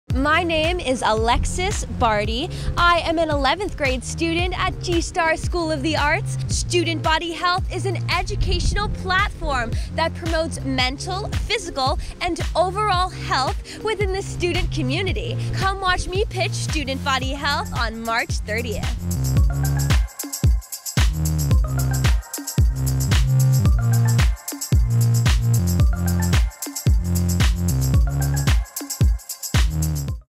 WATCH STUDENTBODYHEALTH AT THE 6TH ANNUAL PHILANTHROPY TANK PALM BEACH COUNTY FINALS EVENT
RSVP NOW for the Philanthropy Tank Palm Beach County Year 6 Finals Event!! It's streaming TONIGHT AT 7!!

Alexis Bardy is an 11th grader at G-Star School of The Arts. She will be pitching StudentBodyHealth. It's a program that aims to boost mental morale and positive health practices for today’s youth during the COVID-19 quarantine by teaching yoga and healthy eating habits.